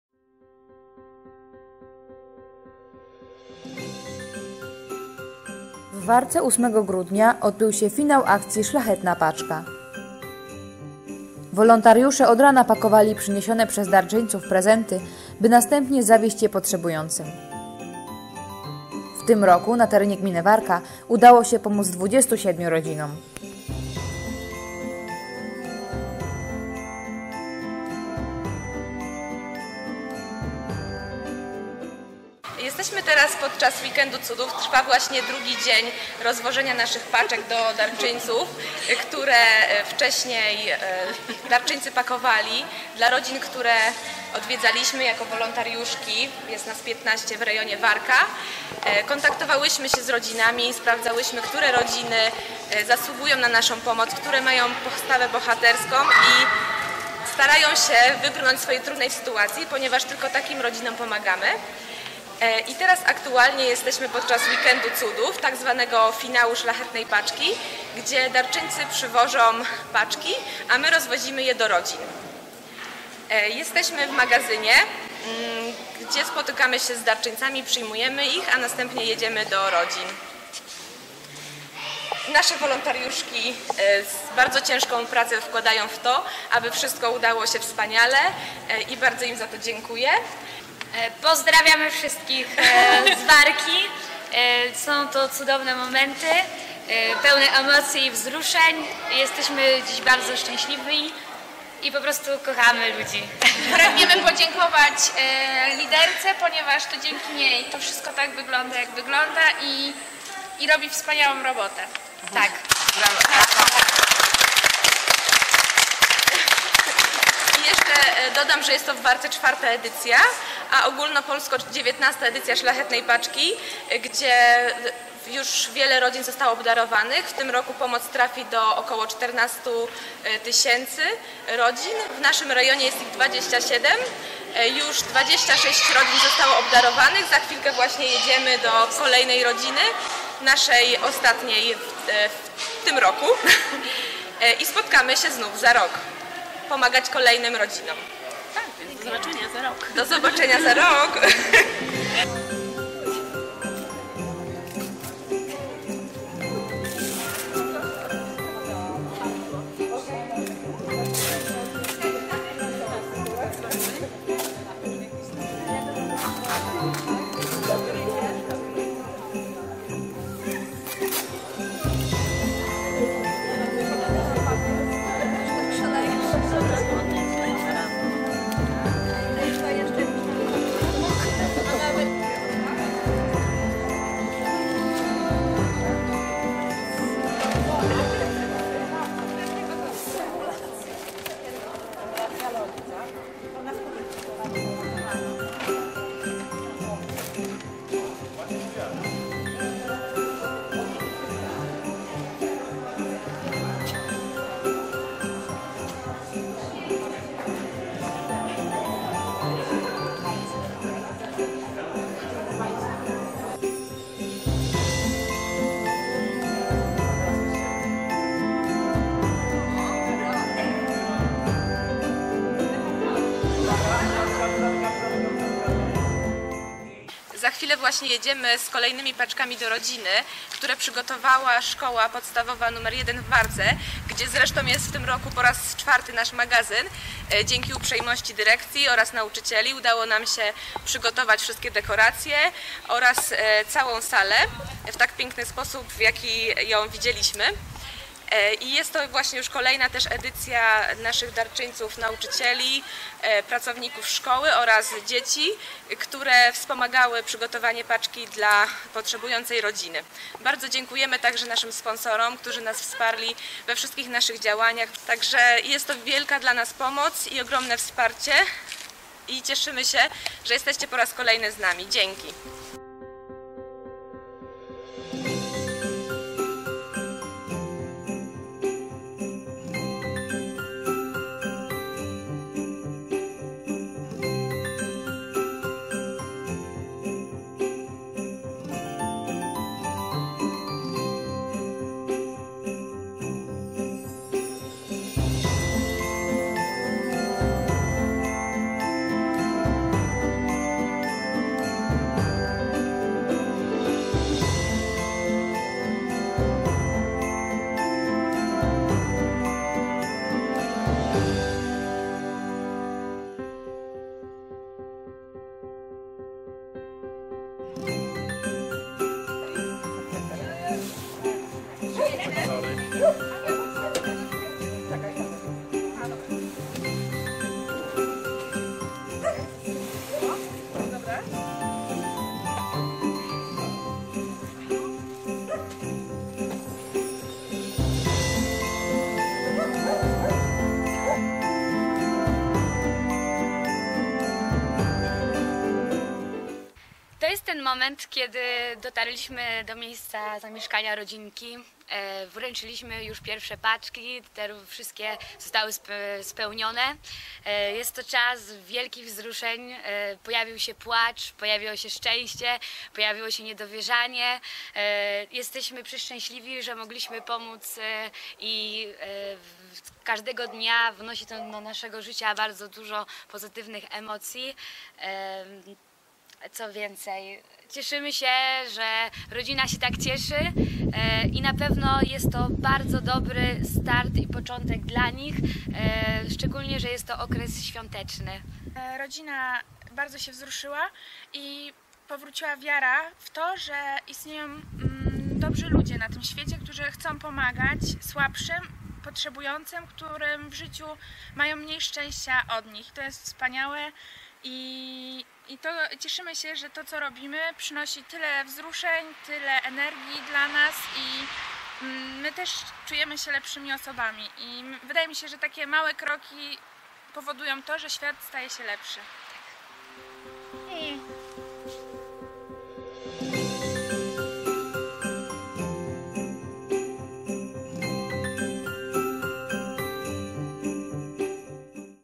Finał Szlachetnej Paczki 2019 w Warce
W Warce 8 grudnia zakończono tegoroczną edycję akcji Szlachetna Paczka. Wolontariusze przyjmowali przyniesione przez darczyńców prezenty, by pięknie je zapakować, a następnie zawieźć do potrzebujących. Tym razem udało się pomóc 27 rodzinom z terenu gminy Warka.